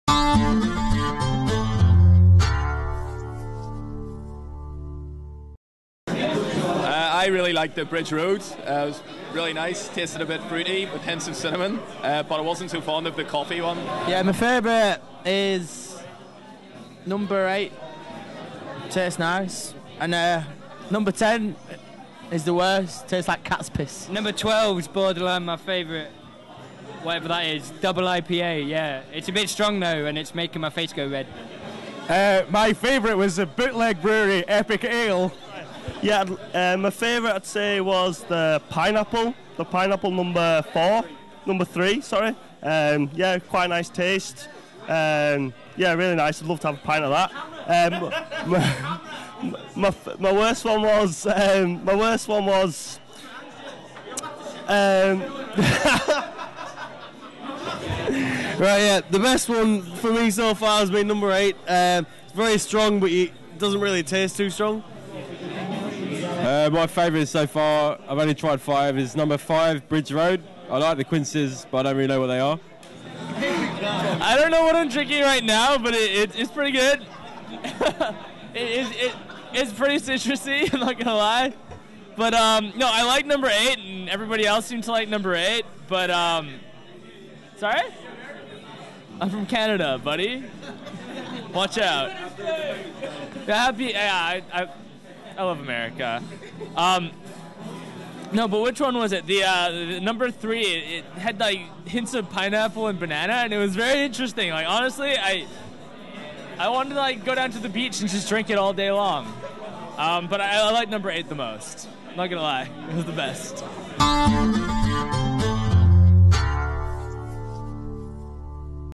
The beer I like is...
This Group of crazy blokes size up which beer they like the most at the American Star  Spangled beer spectapular at the local taphouse in St Kilda.